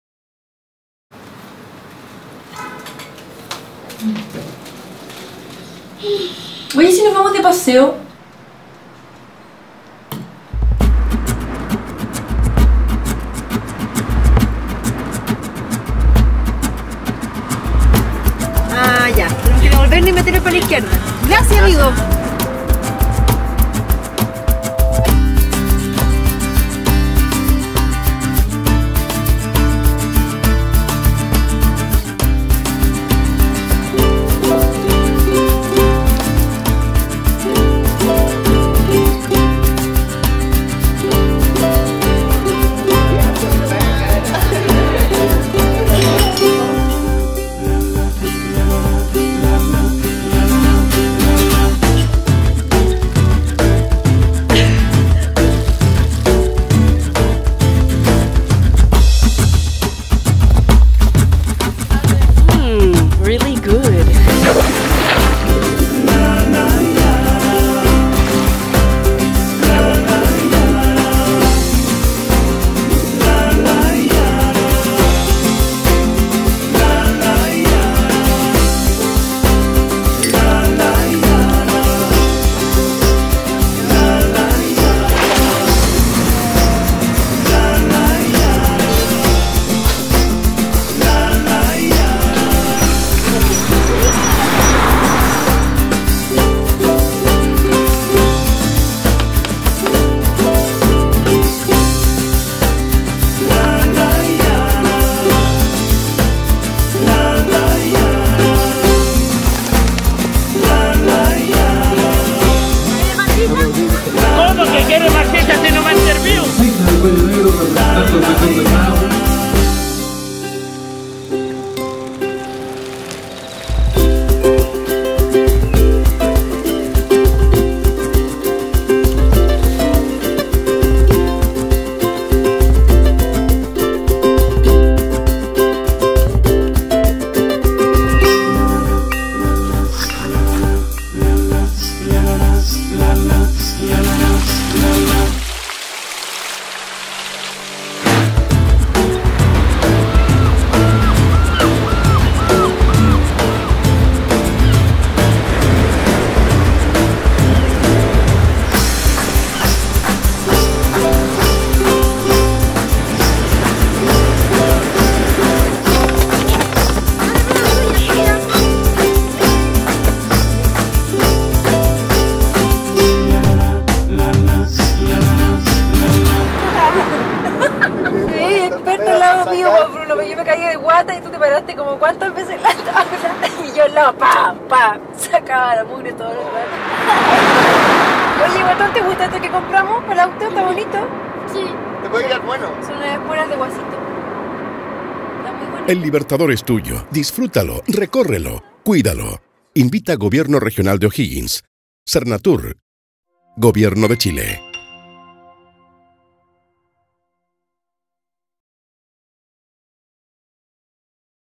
EL LIBERTADOR A UN PASO
Conoce los atractivos turísticos que tiene la Región del Libertador, elige tu destino: playa,. valle o cordillera... Seguro te motivas a venir!!.

Video promocional de los atractivos de la Región del Libertador General Bernardo O'Higgins realizado por la dirección regional del Servicio Nacional de Turismo.